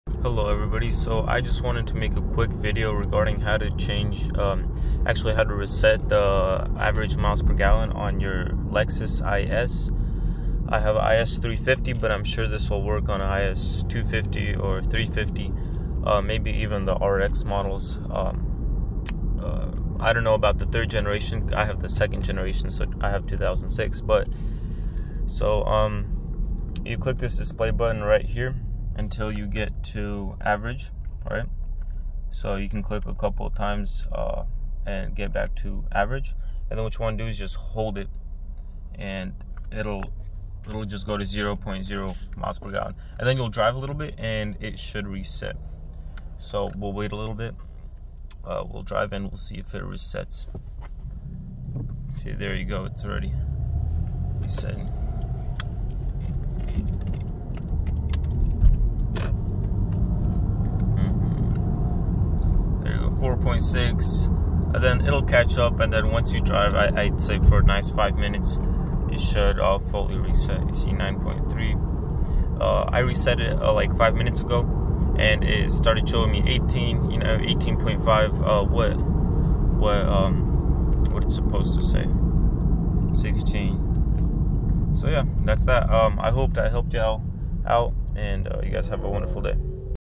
Reset mpg for Lexus Is 250/350
If your mpg is off or is acting up then try reseting it, might help.